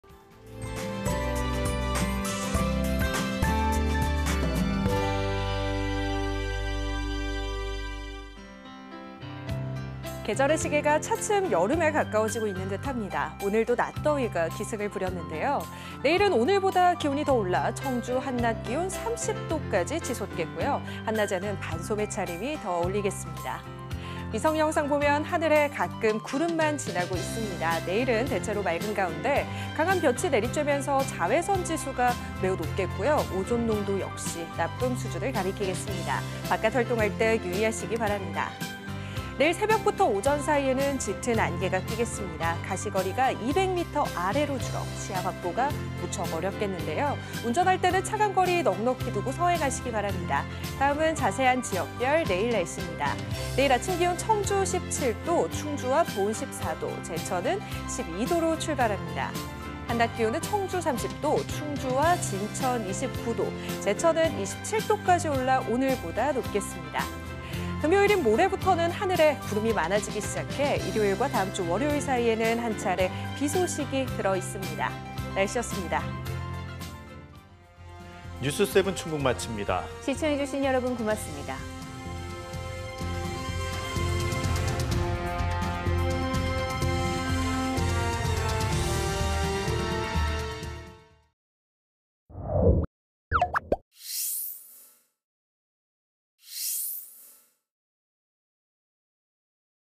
[날씨] 충북 내일도 낮 더위 기승…출근길 짙은 안개 / KBS 2024.05.22.
계절의 시계가 차츰 여름에 가까워지고 있는듯합니다.

 오늘도 낮 더위가 기승을 부렸는데요.

 내일은 오늘보다 기온이 더 올라 청주 한낮기온 30도까지 치솟겠고요.

 한낮에는 반소매 차림이 더 어울리겠습니다.

 위성영상 보면 하늘에 가끔 구름만 지나고 있습니다.

 내일은 대체로 맑은 가운데 강한 볕이 내리쬐면서 자외선 지수가 매우 높겠고요.

 오존농도도 나쁨 수준을 가리키겠습니다.

 바깥 활동 할 때 유의하시기 바랍니다.

 내일 새벽부터 오전 사이에는 짙은 안개가 끼겠습니다.

 가시거리가 200m 아래로 줄어 시야 확보가 어렵겠는데요.

 운전할 때는 차간 거리 넉넉히 두고 서행하시기 바랍니다.

 다음은 자세한 지역별 내일 날씨입니다.

 내일 아침 기온 청주 17도, 충주와 보은 14도, 제천은 12도로 출발합니다.

 한낮 기온은 청주 30도, 충주와 진천 29도, 제천은 27도까지 오르며 오늘보다 높겠습니다.

 금요일인 모레부터는 하늘에 구름이 많아지기 시작해 일요일과  다음 주 월요일 사이에는 한차례 비 소식이 들어있습니다.